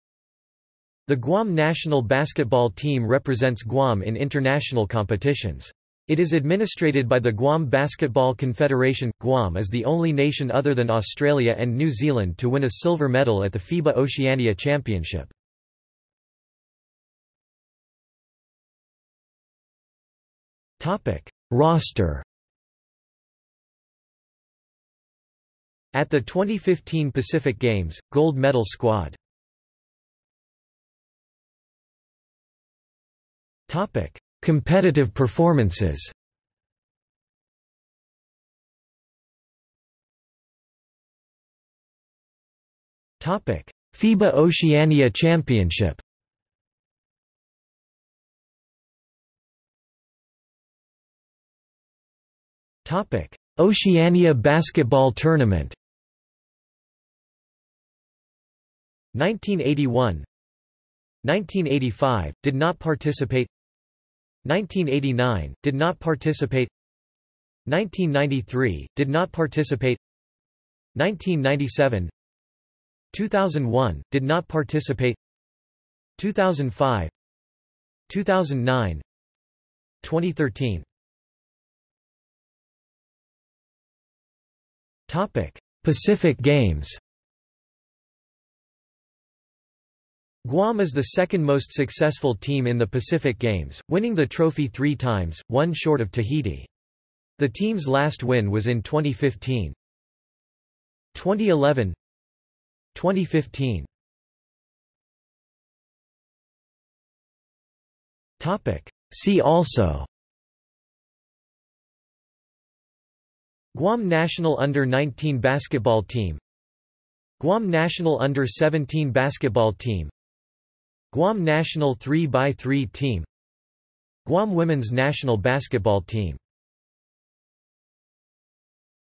Guam national basketball team | Wikipedia audio article
00:00:17 1 Roster
00:00:30 2 Competitive performances
00:00:39 2.1 FIBA Oceania Championship
00:00:49 2.2 Oceania Basketball Tournament
00:01:21 2.3 Pacific Games
00:01:44 3 See also

- Socrates

SUMMARY
The Guam national basketball team represents Guam in international competitions. It is administrated by the Guam Basketball Confederation.Guam is the only nation other than Australia and New Zealand to win a silver medal at the FIBA Oceania Championship.